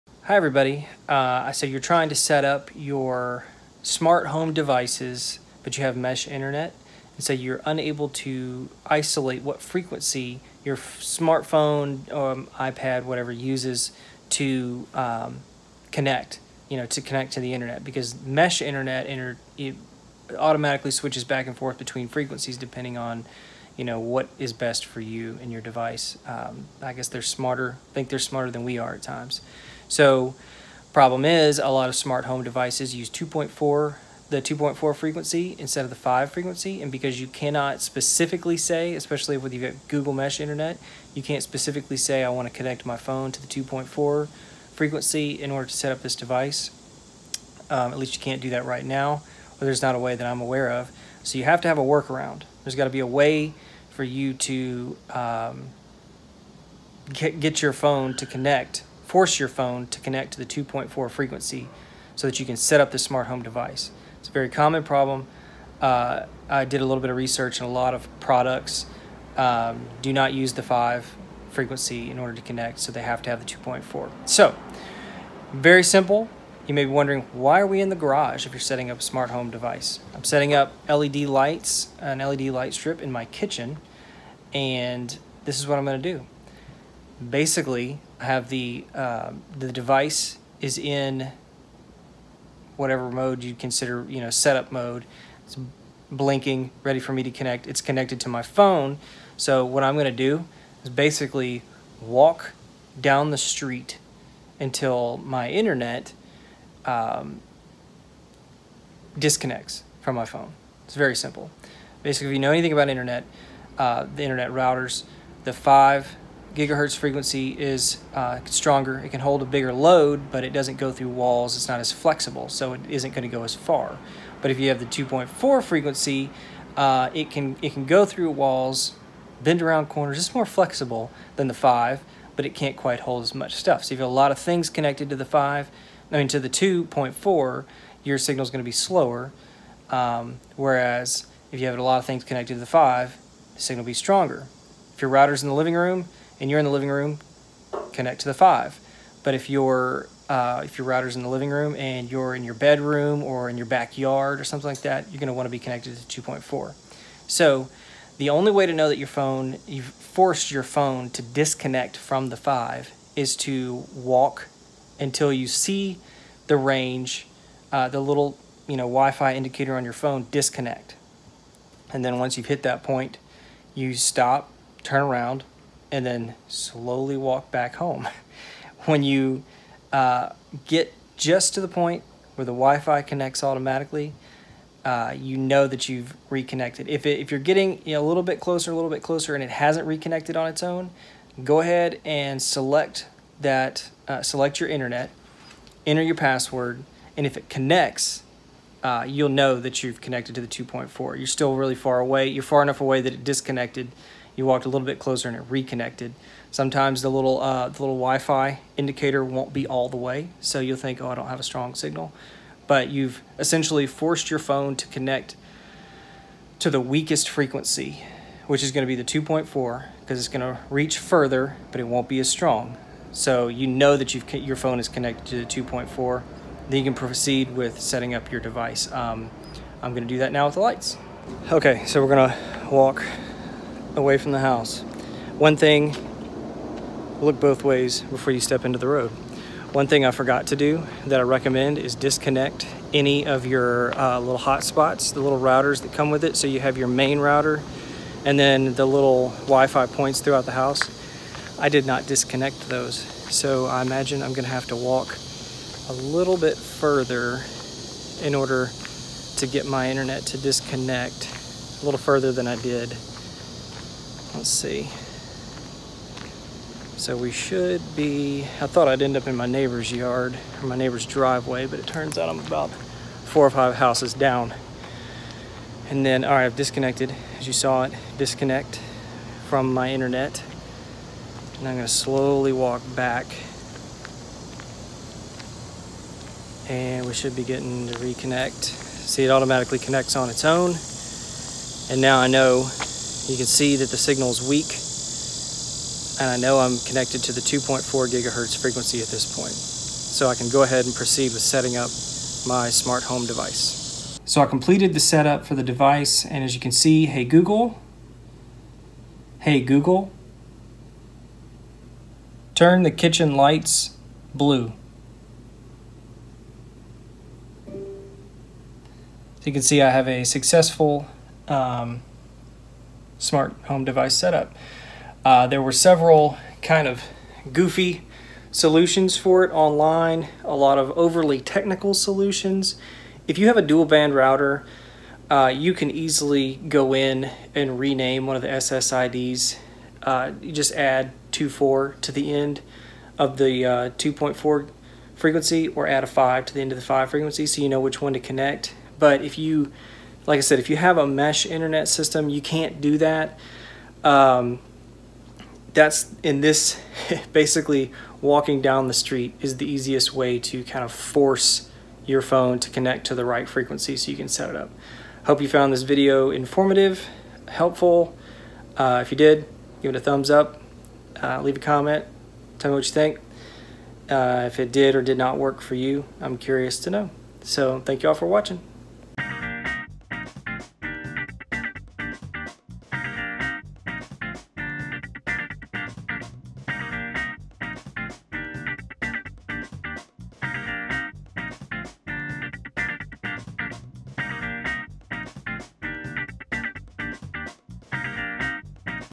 How To Connect Your 2.4 GHz Smart Home Device to Mesh Internet
This video is a little different from our usual stuff. We're posting a product review video on Tuesday and this particular product required me to do a little research on how to connect smart home devices to the internet is you've got mesh internet. I hope you find this little trick to be helpful.

Camera - iPhone8
Edited - Adobe Premiere Elements 18